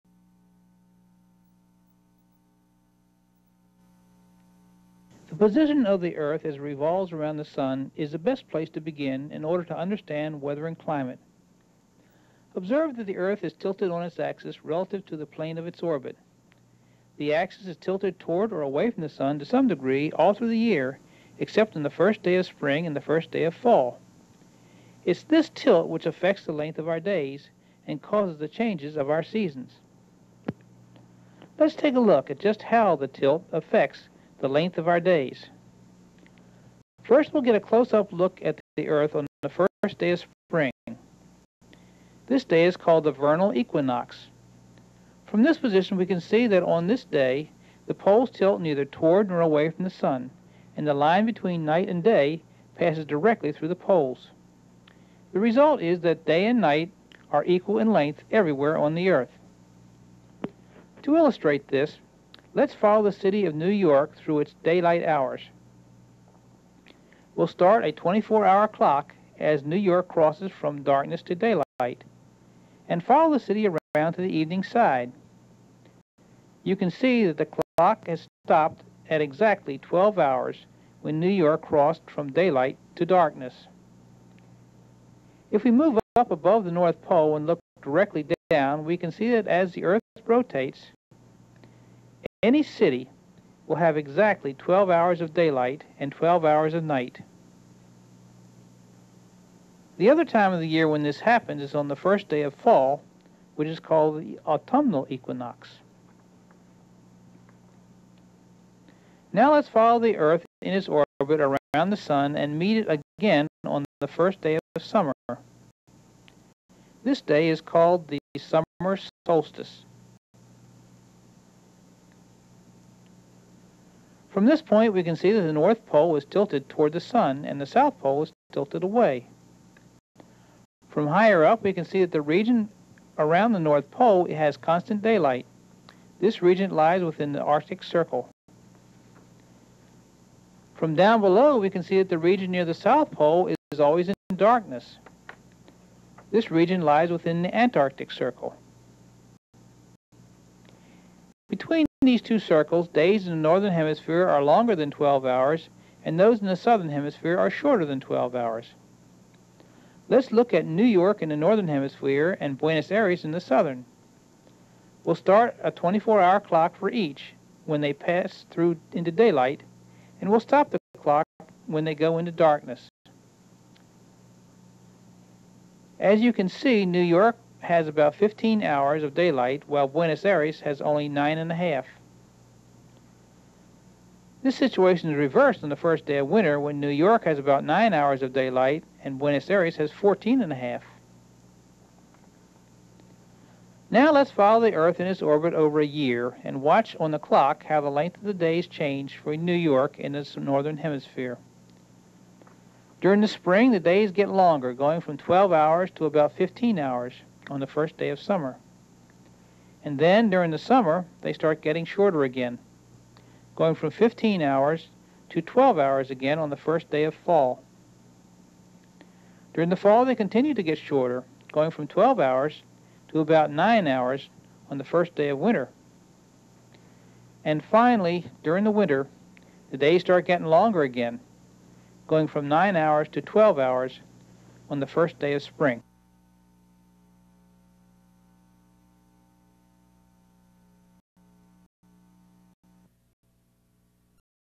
Cycles of the Atmosphere: Module 2
Narrated movie describing how the Earth's tilt affects the length of days and triggers the changing of the seasons.
Scientific Visualization Studio